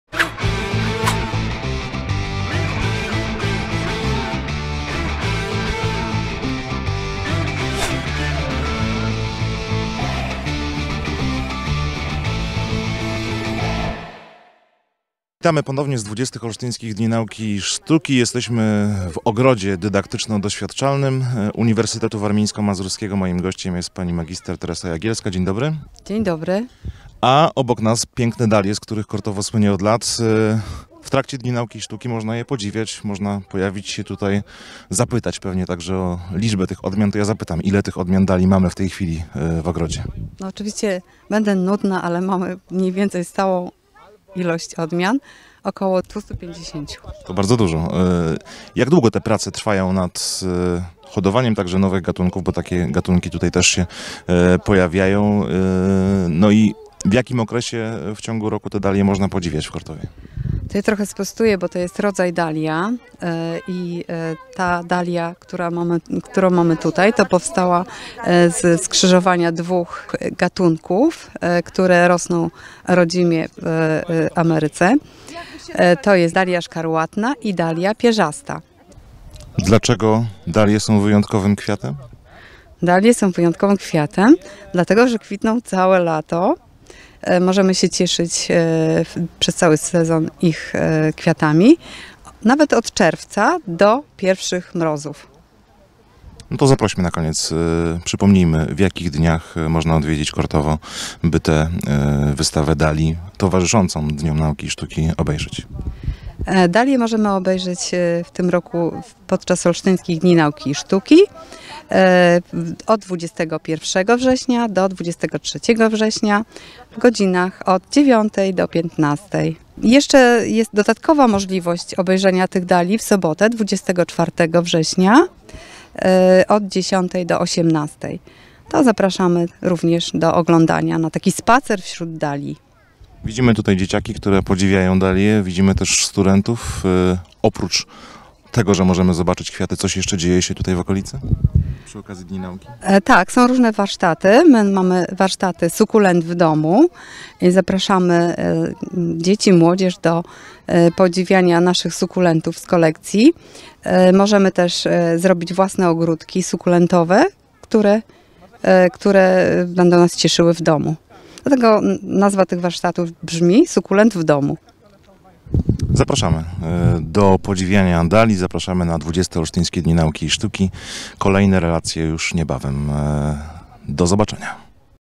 XX ODNiS- ogrody UWM
150 odmian dalii- najdłużej kwitnących kwiatów w Polsce. Rozmowa z mgr Teresą Jagielską z Wydziału Biologii i Biotechnologii UWM.